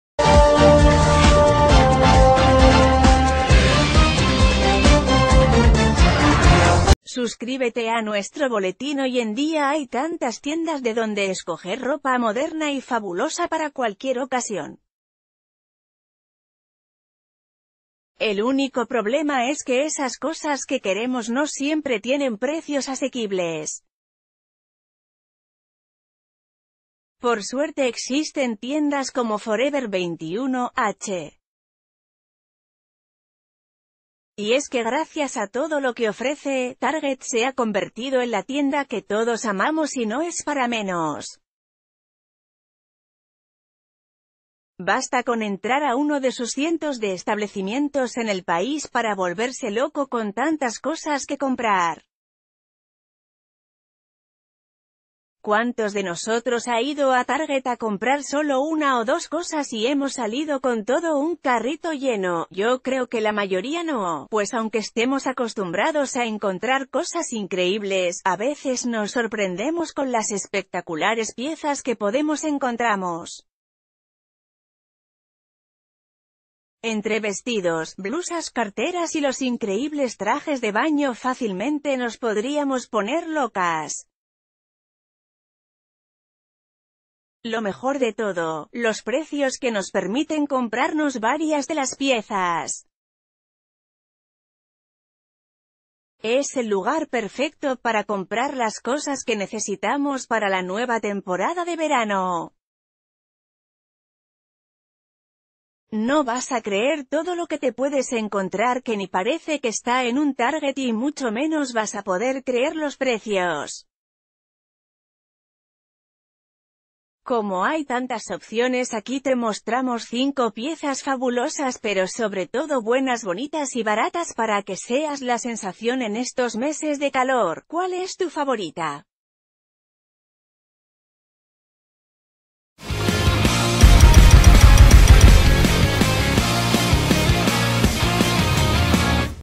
No vas a creer que estas fabulosas piezas las puedes encontrar en Target
El único problema es que esas cosas que queremos no siempre tienen precios asequibles. Por suerte existen tiendas como Forever 21, H&M y por su puesto la favorita de todos, Tar...